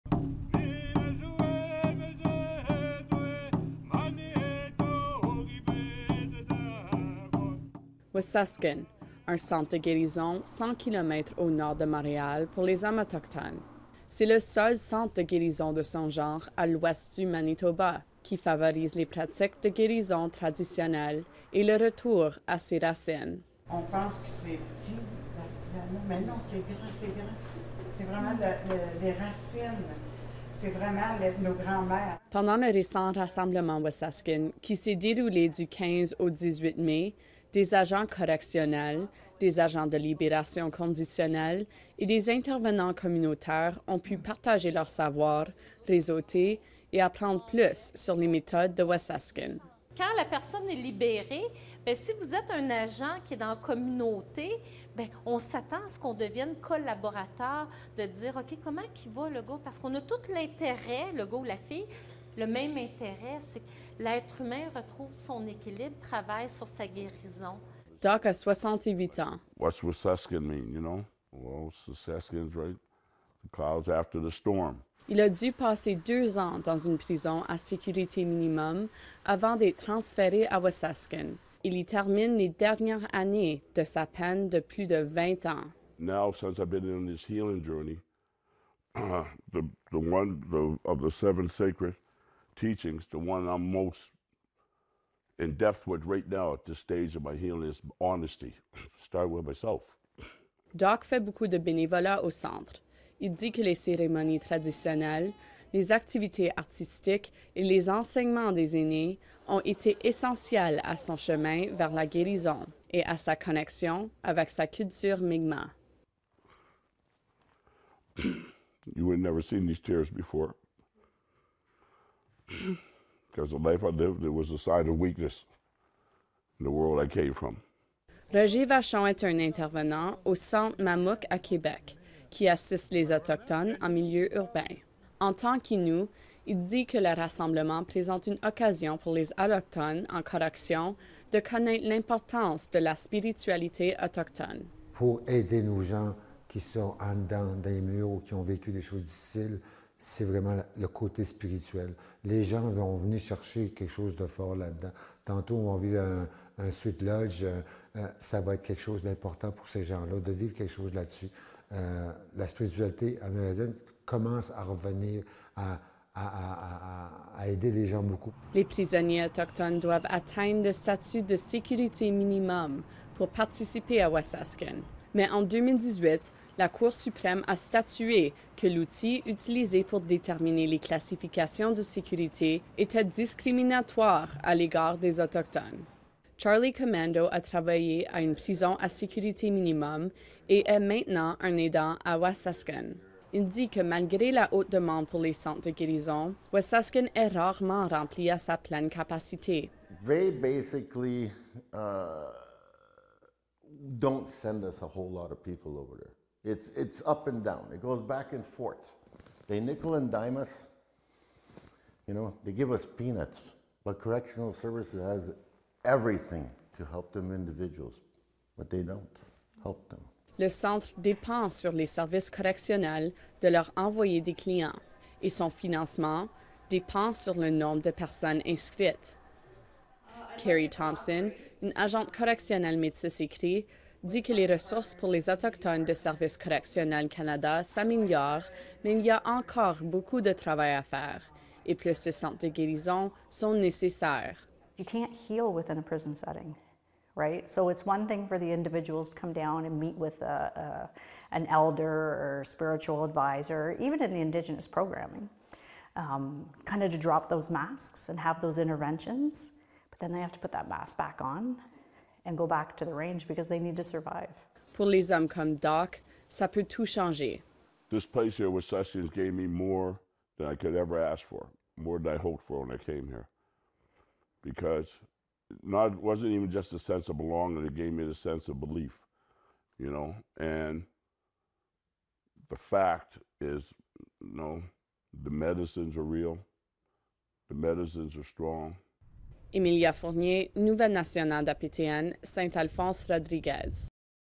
Waseskun : Les hommes autochtones en milieu carcéral, une histoire de guérison
Nous étions sur le terrain à Saint-Alphonse-Rodriguez où un centre de guérison autochtone au Québec a rassemblé ceux qui travaillent avec les hommes autochtones incarcérés afin d’échanger sur l’importance des centres de guérison et apprendre à les accompagner au mieux.